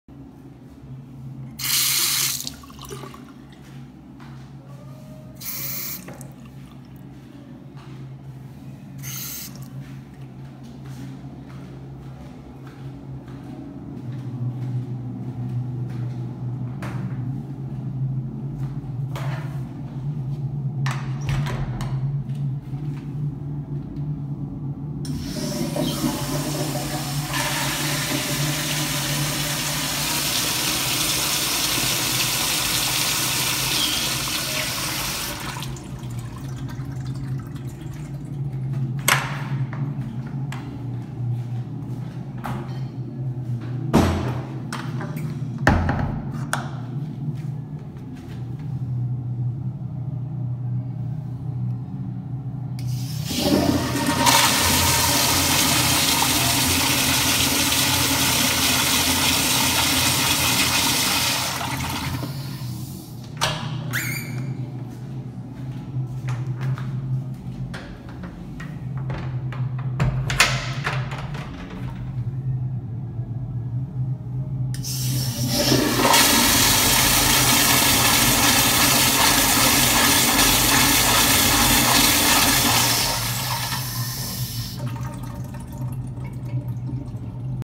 Kohler Fixtures at Horse and Plow
This is a Restaurant at/ in The American Club in Kohler, Wisconsin.